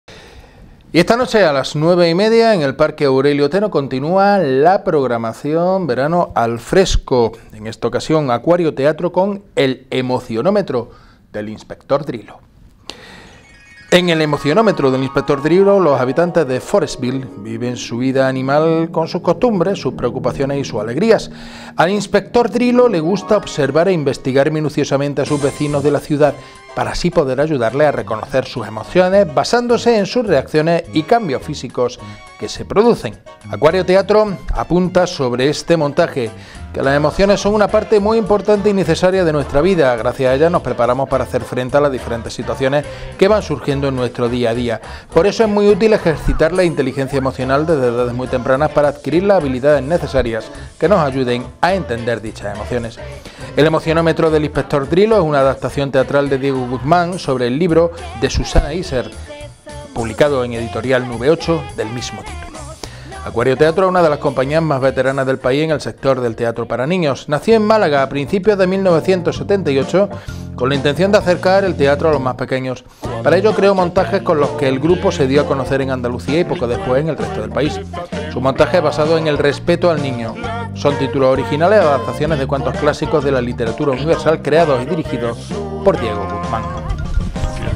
La compañía Acuario Teatro nos trae la historieta "El emocionómetro del inspector Drilo"TOR DRILO
El Auditorio del Parque Aurelio Teno acoge la puesta de escena de la historieta El emocionómetro del inspector Drilo”, de la compañía Acuario Teatro, dentro de la programación Al Fresco.